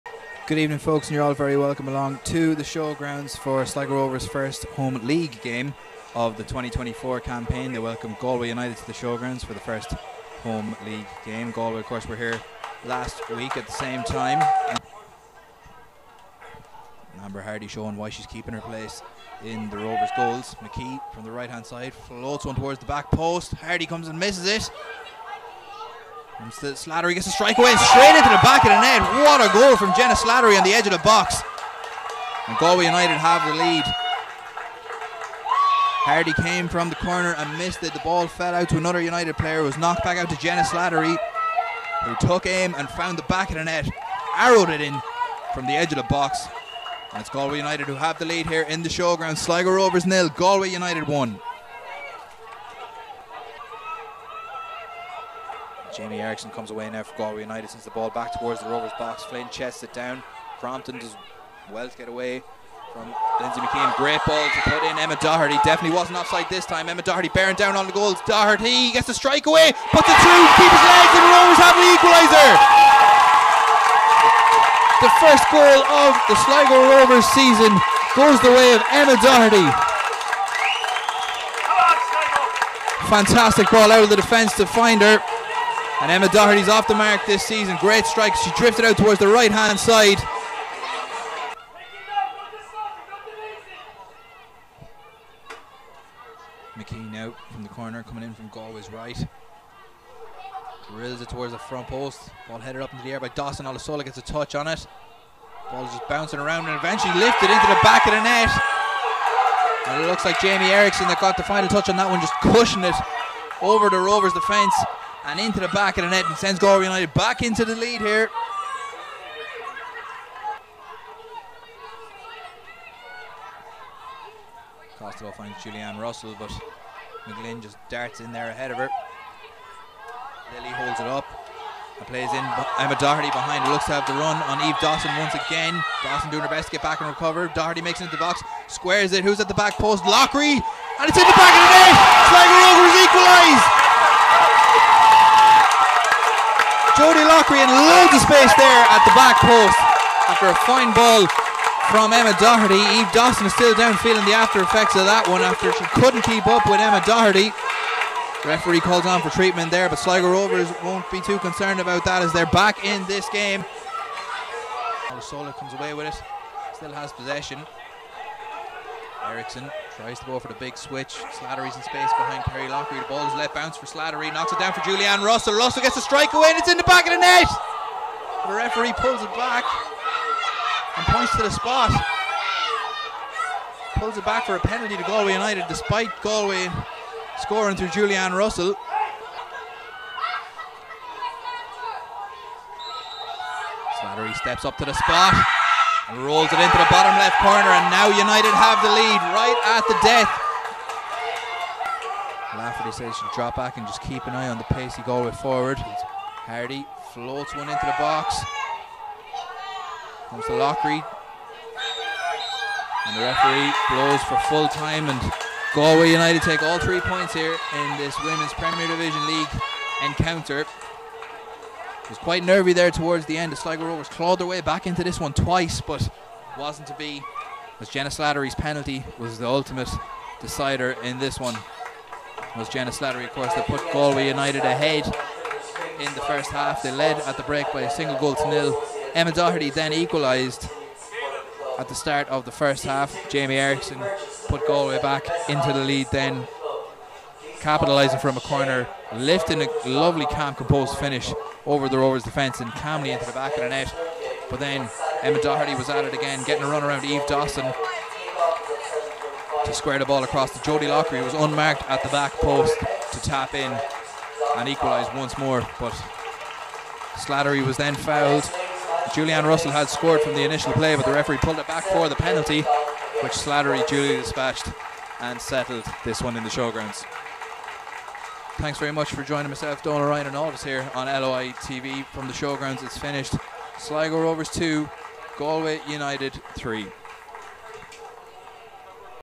SSE Airtricity Women’s Premier Division Round 3 | Sligo Rovers 2-3 Galway United | All the goals
All the goals from Galway United's dramatic win over Sligo Rovers.